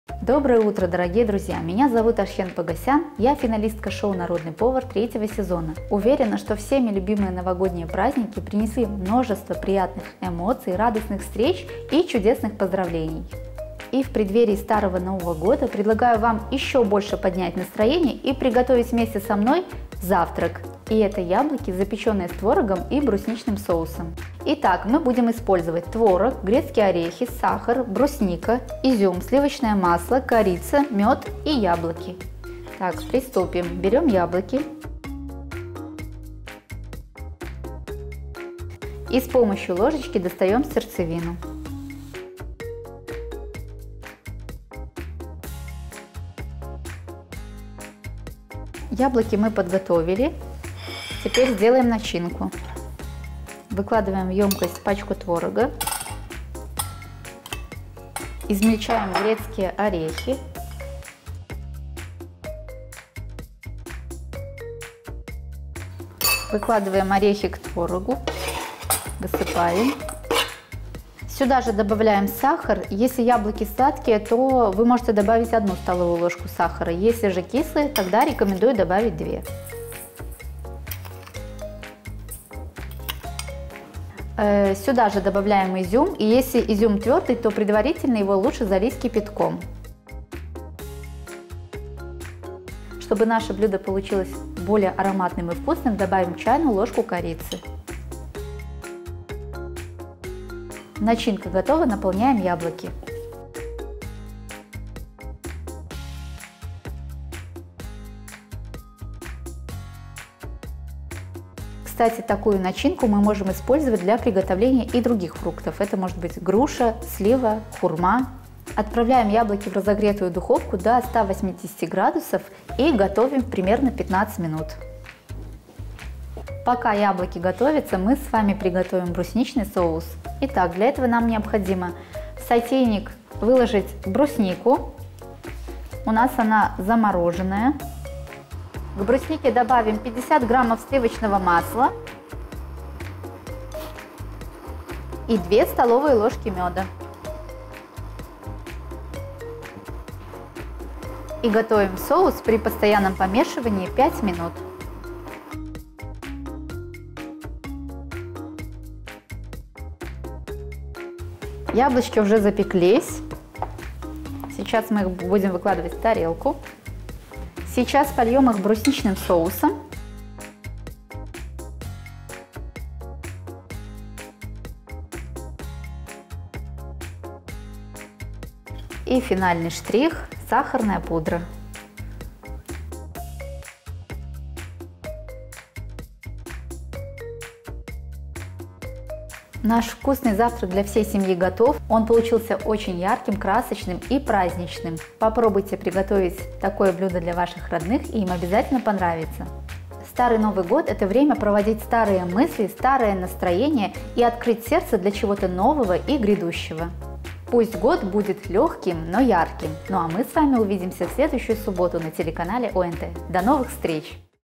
ЗАПЕЧЕННЫЕ ЯБЛОКИ – с творогом и брусничным соусом | Субботний завтрак
Старый Новый год – отличный повод собраться всей семьей за одним столом! Уютная, волшебная, домашняя атмосфера в эти праздничные дни – гарантирована! А для того, чтобы подкрепить настроение, приготовим оригинальный завтрак. Смело повторяйте рецепт на своей кухне и делитесь результатом в комментариях!